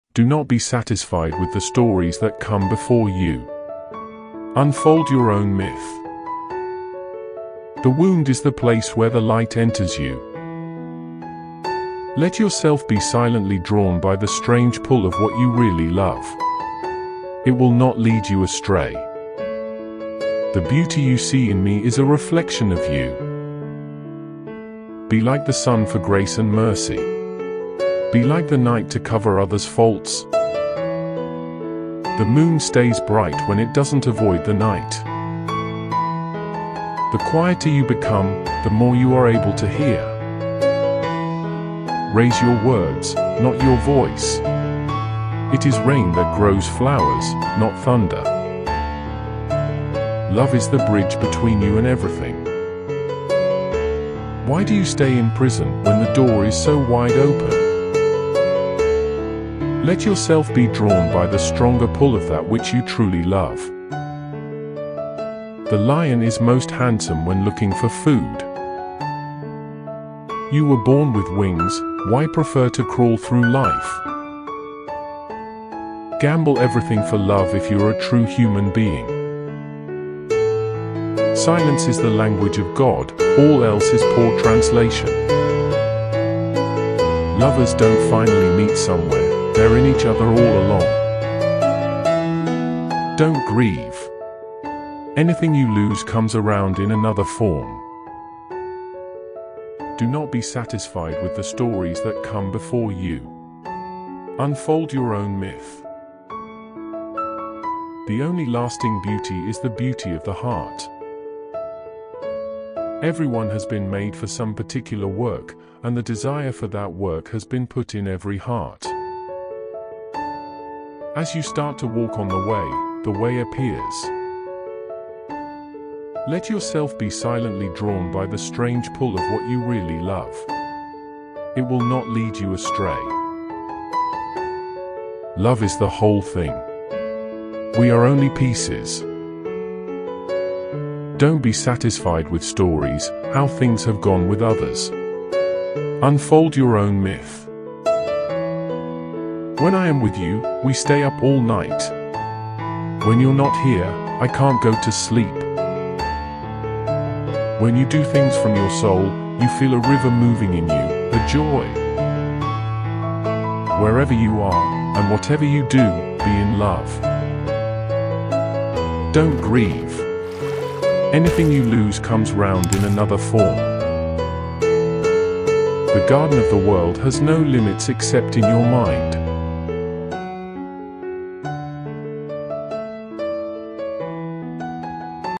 Molana Rumi Quotes | Molana Rumi Kalam
Molana Jalal ad-Din Rumi, commonly known as Rumi, was a 13th-century Persian poet, mystic, and theologian who became one of the most celebrated and influential figures in the history of Persian literature. He was born on September 30, 1207, in Balkh (in present-day Afghanistan) and died on December 17, 1273, in Konya (in present-day Turkey).